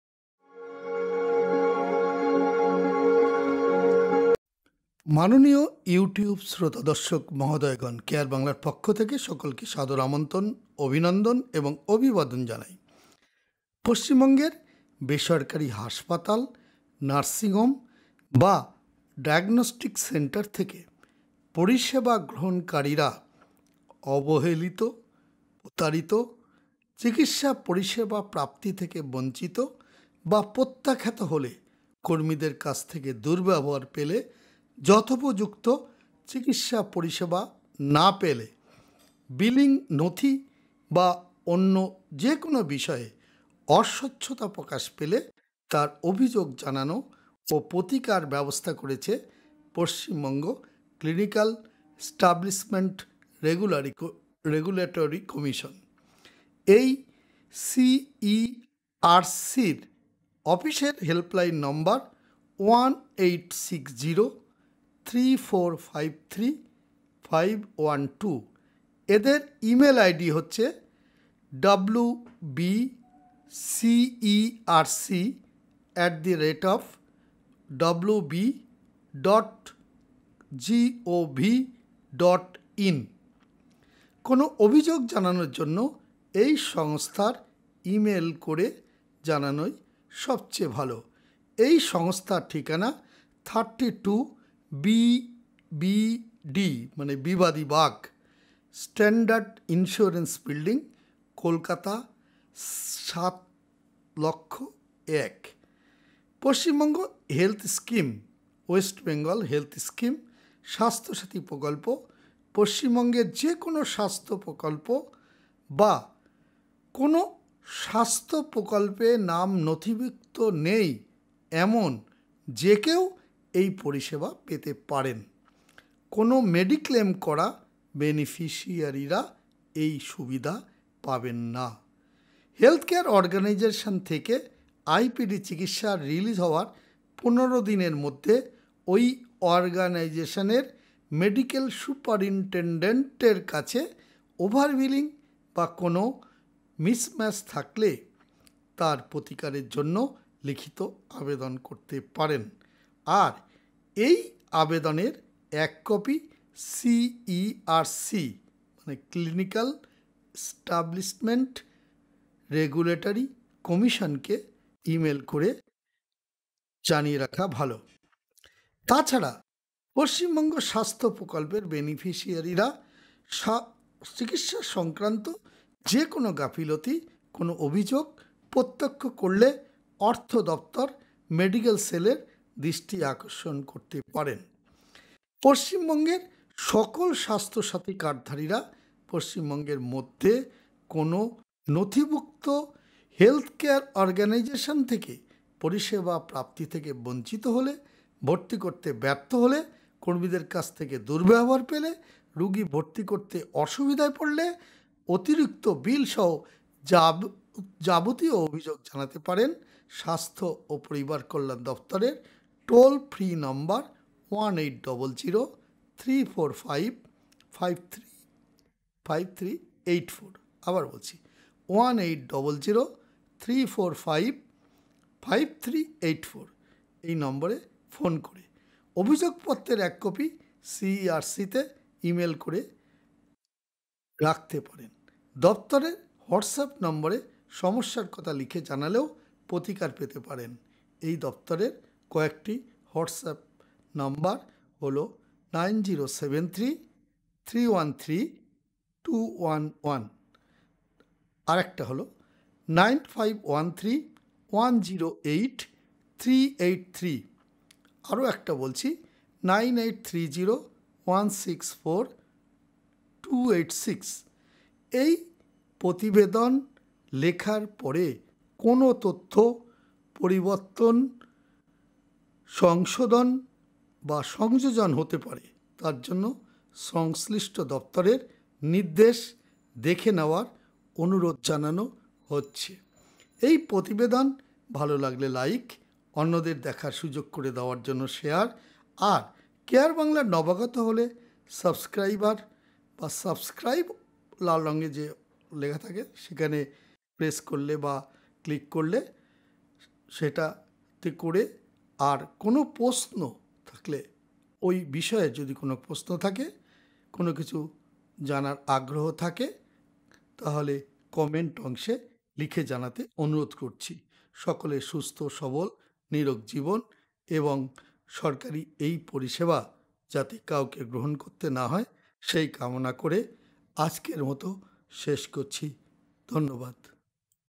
স্বাস্থ্য পরিষেবায় গাফিলতির অভিযোগ জানানো ও ক্ষতিপূরণ আদায় !!
1. Compensation Against wrong treatment.
2. Complain against misbehavior of Health Care Organisation personnel.
3. Complain against Hospital, Nursing Home & Diagnostic  Centre.
4. Complain against Over-billing & unnecessary procedures from HCO.
4. Arbitrator of Swastha Sathy, West Bengal health scheme, Non - Enroled individual person.
6. West Bengal health scheme. WBHS